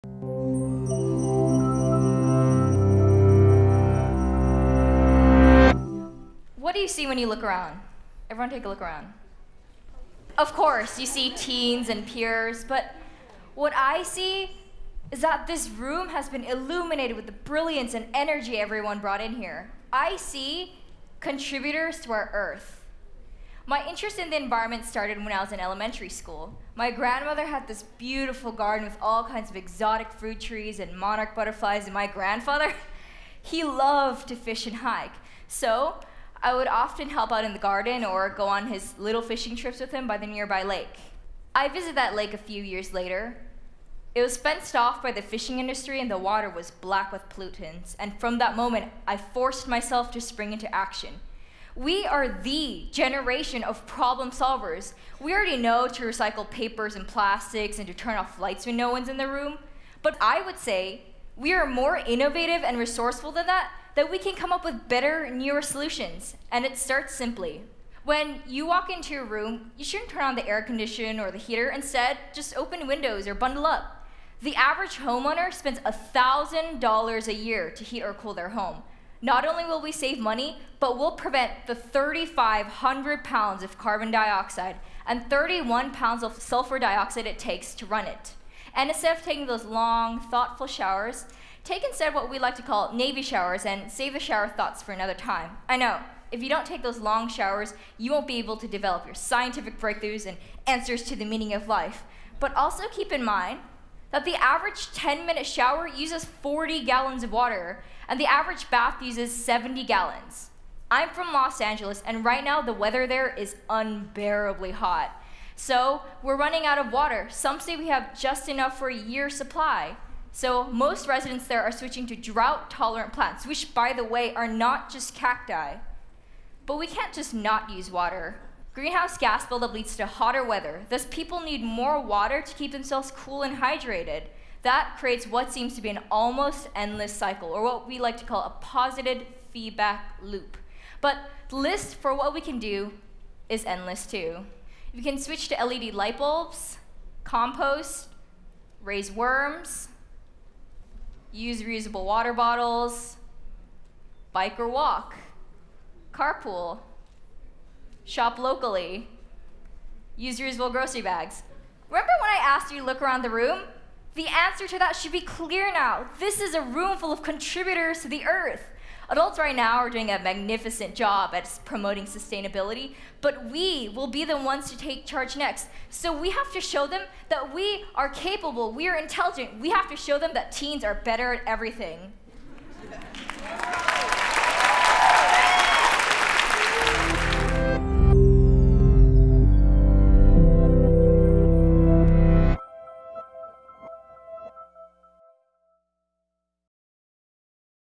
A few tips on how this generation can change global warming | Chelsea Ha | North High School
Global warming, pollution, and other environmental hazards are ever-expanding and affecting our world. In this talk,  Chelsea Ha explains how these issues can affect us in our daily lives, and shares easy ways we can help fix the problem.

To learn more about TED-Ed Clubs or to start your own club, go